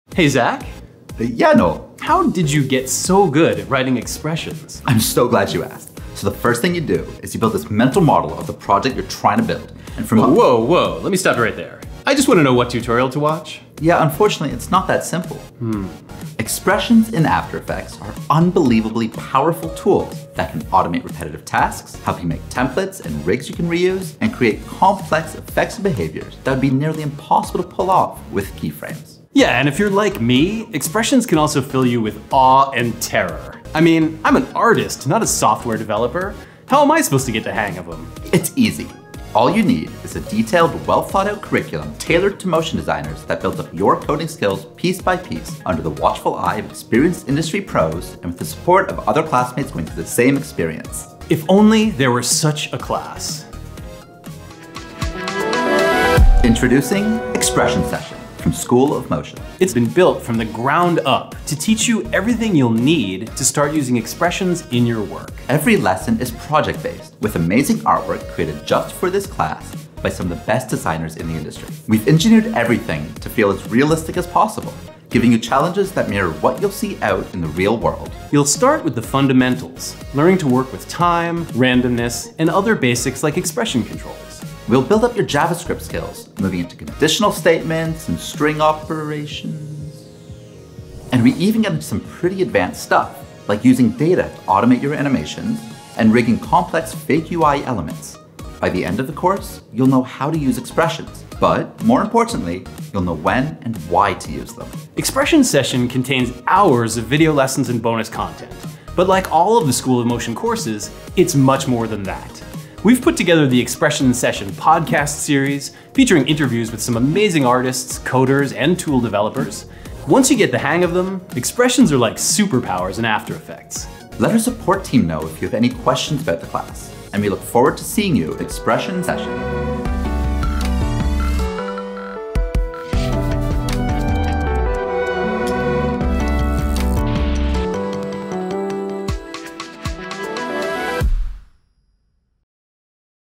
Expression Session | Master Expressions in After Effects
Expression Session will teach you how to approach, write and implement expressions in After Effects. Over the course of 12 weeks, you'll go from rookie to seasoned coder. By the end of this course, you will have all of the foundational knowledge you need to be able to write, understand, and use Expressions in After Effects to improve your daily workflow!